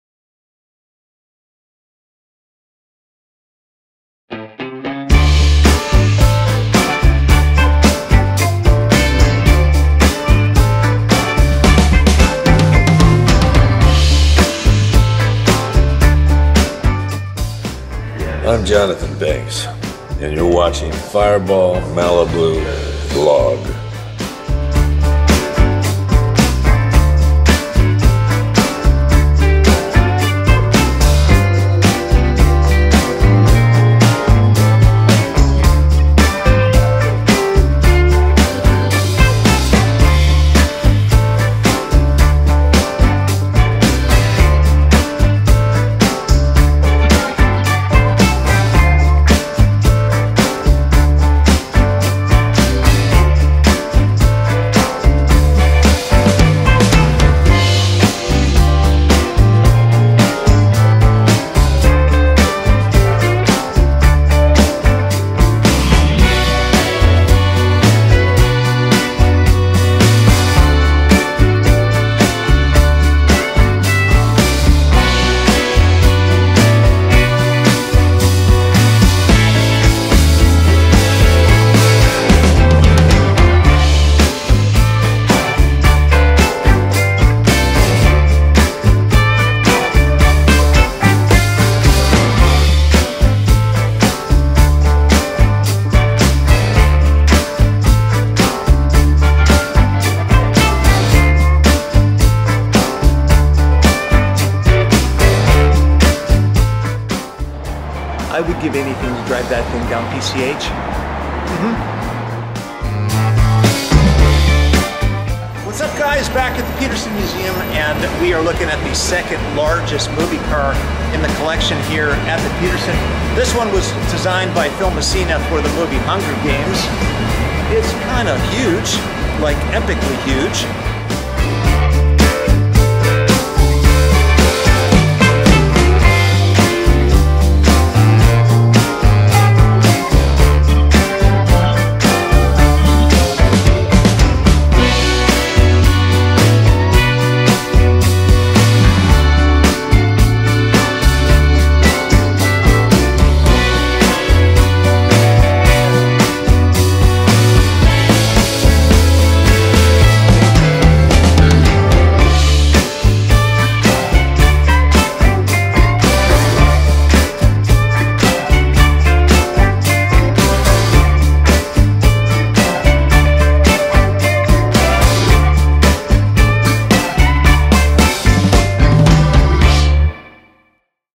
HUNGER GAMES PEACEKEEPER MOVIE CAR - 3MINUTE CUSTOMS
- HUNGER GAMES PEACEKEEPER MOVIE CAR - 3MINUTE CUSTOMS - After some shenanigans in Malibu, Fireball realizes he needs a new ride to battle PCH.